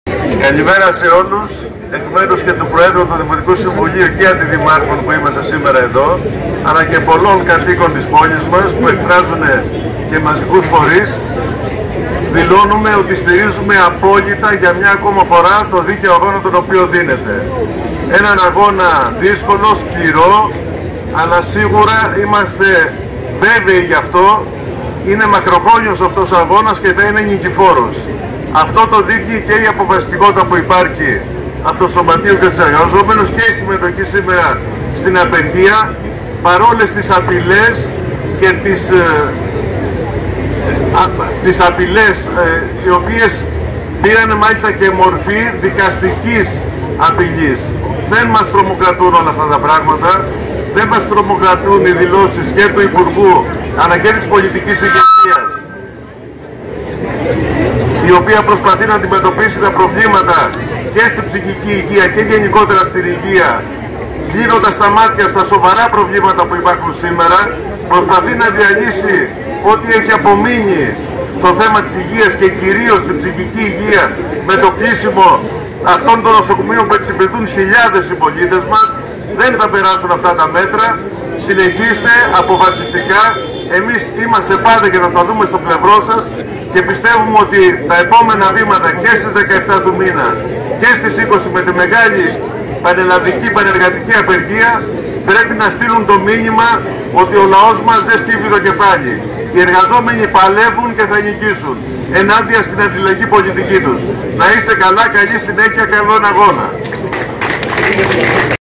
Ο Μ. Σελέκος στην απεργιακή συγκέντρωση στο ΨΝΑ
«Δηλώνουμε ότι στηρίζουμε απόλυτα για μια ακόμα φορά τον δίκαιο αγώνα τον οποίο δίνεται. Ένα αγώνα δύσκολο, σκληρό και μακροχρόνιο αλλά σίγουρα είμαστε βέβαιοι γι' αυτό, θα είναι ένας νικηφόρος», υπογράμμισε στον χαιρετισμό του στη συγκέντρωση ο *Μιχάλης Σελέκος,* δήμαρχος Χαϊδαρίου.